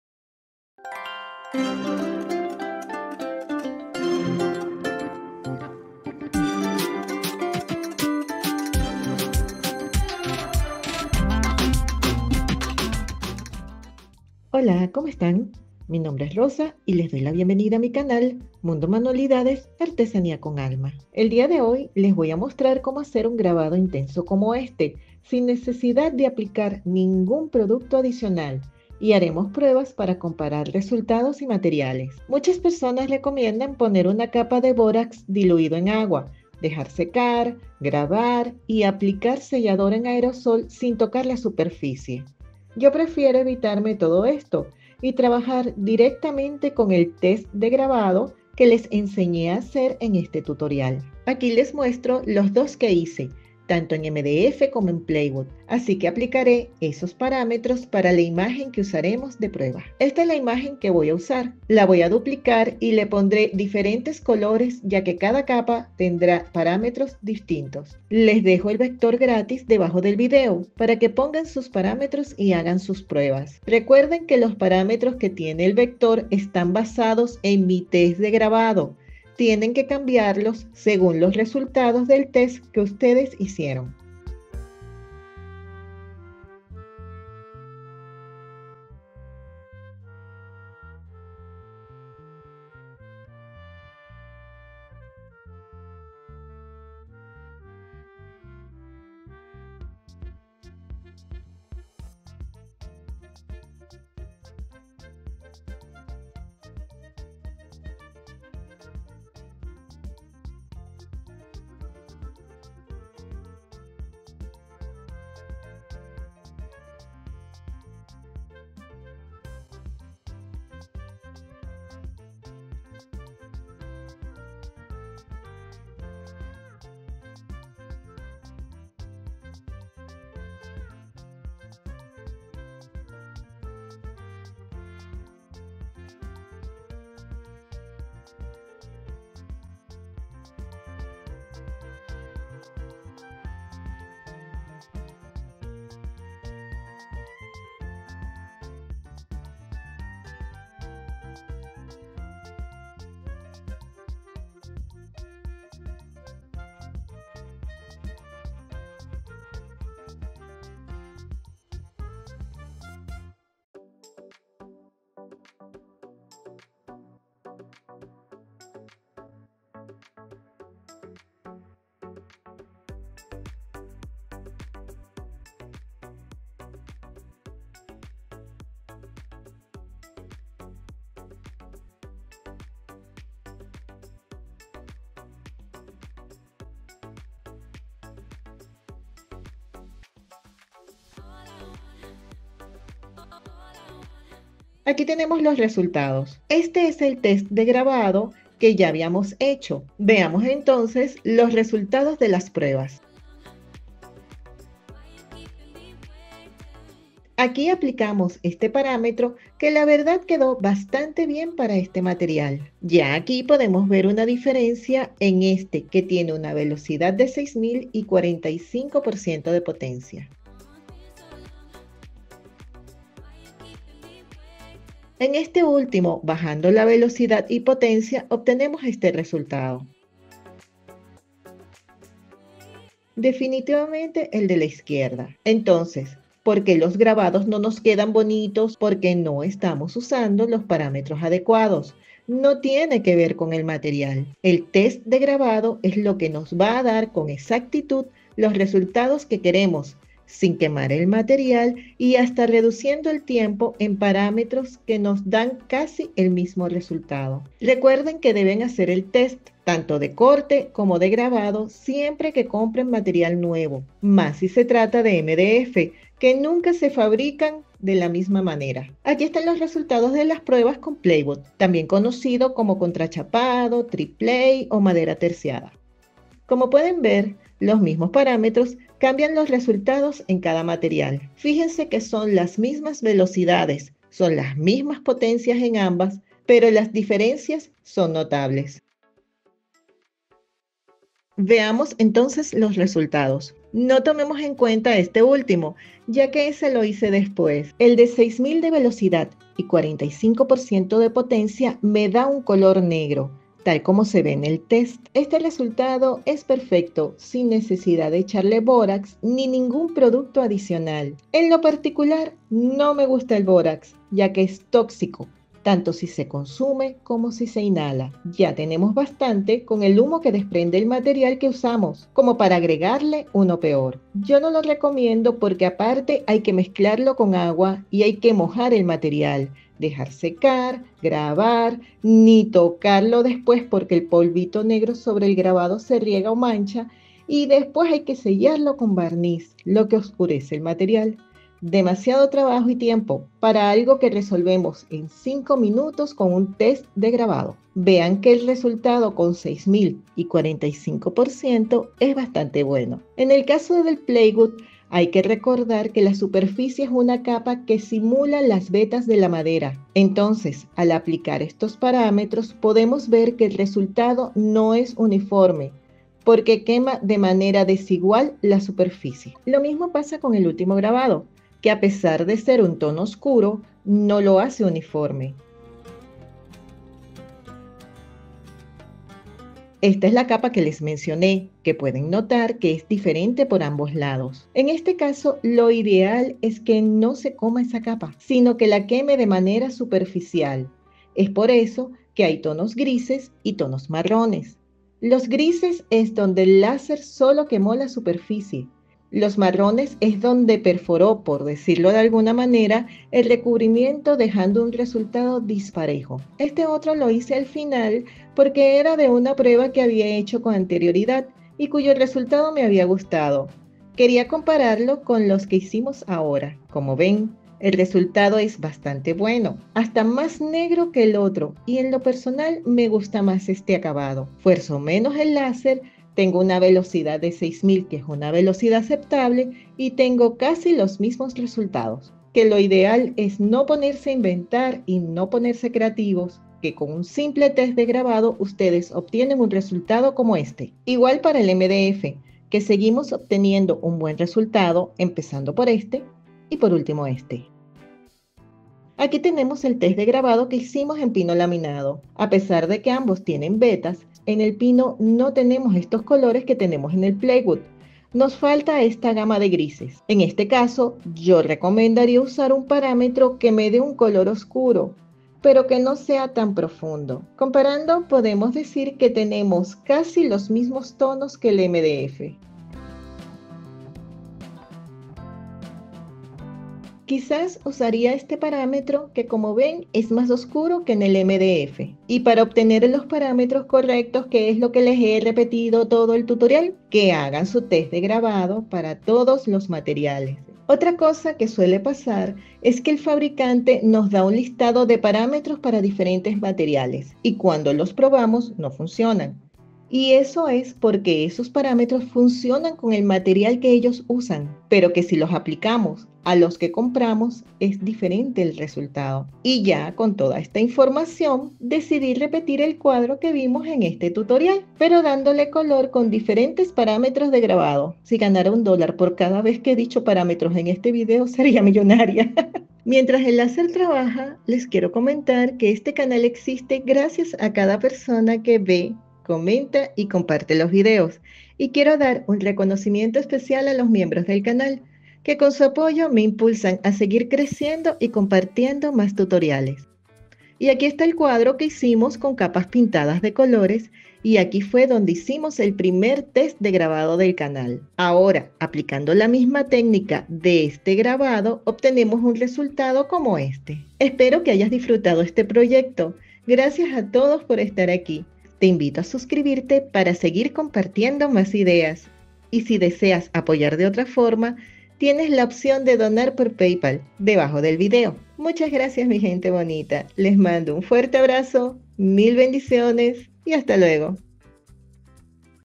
‼️⚠️Evita errores: Cómo conseguir grabados perfectos con tu Cortador Láser de Diodo
Hola, mi gente bonita 🌸
En este tutorial te muestro cómo lograr un grabado láser intenso y uniforme en MDF y  Plywood, sin necesidad de usar bórax ni ningún producto adicional.

Hacemos varias pruebas comparando materiales y parámetros, usando el test de grabado que ya les enseñé a realizar. Vas a ver cómo cambia el resultado según la potencia, velocidad y tipo de madera, y lo más importante: cómo encontrar los parámetros correctos para tus proyectos sin dañar el material ni perder tiempo.

Si usas tu cortador láser para emprender, este video es perfecto para ti porque aprenderás a optimizar tus grabados y reducir costos.